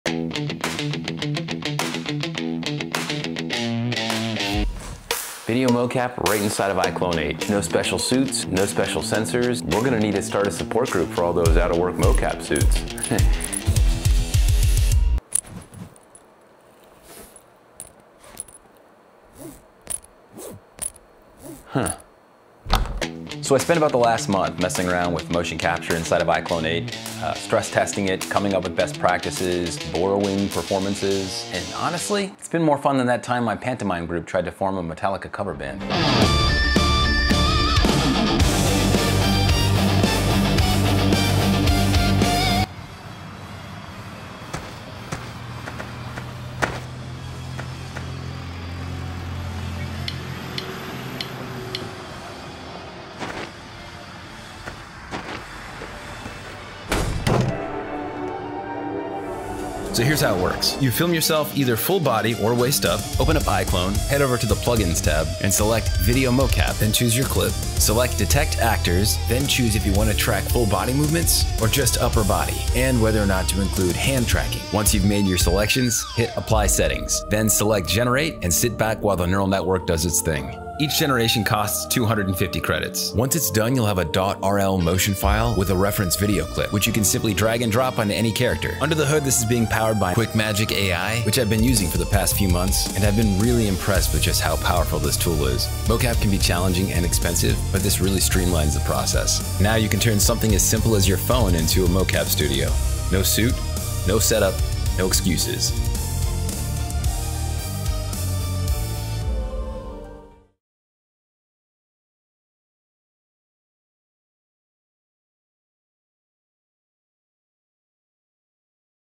iClone Video Mocap: Fast, Studio-Free Capture for Indie Creators | iClone 8
Transform your living room into a mocap studio with iClone Video Mocap.

Using only a single camera, indie creator @NickShaheen  captures clean, natural performances in minutes — no suits, no sensors, no calibration. From stylized characters to full band animations performed entirely by himself, Nick shows how fast and flexible mocap can be inside iClone.

Whether you're animating previs, stylized shorts, or fast-turnaround ideas, iClone Video Mocap gives you a studio-free workflow that fits right into any home setup. One camera. One clip. Instant 3D motion.